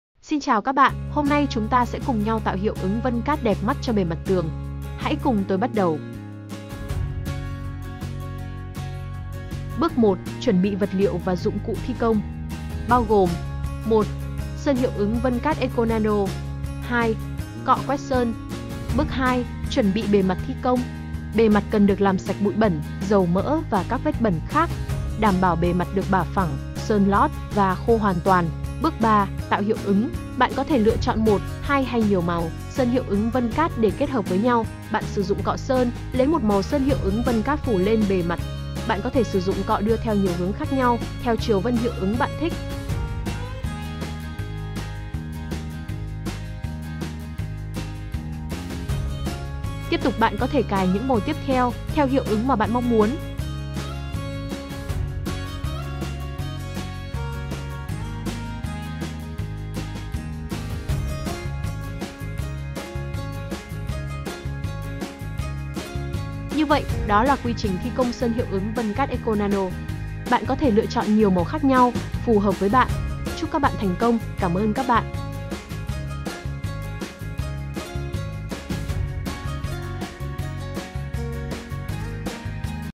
HƯỚNG DẪN THI CÔNG SƠN HIỆU ỨNG VÂN CÁT ECONANO
Một trong những điểm độc đáo của Sơn Hiệu ứng là Với việc tạo vân bằng các dụng cụ đa dạng giống tự nhiên và màu sơn Phong phú sẽ biến mỗi bức tường thành 1 tác phẩm nghệ thuật mang tính độc bản, mà ngay cả người thợ làm ra tác phẩm đó, cũng ko thể tái hiện lại dc bức tường thứ hai có vân và màu giống hệt bức tường đã làm trước đó.
Điều này khiến sơn hiệu ứng, sơn nghệ thuật sẽ trở thành xu hướng trong nghành kiến trúc. Khi các chủ nhân muốn thể hiện cá tính hay nét riêng có trong không gian của mình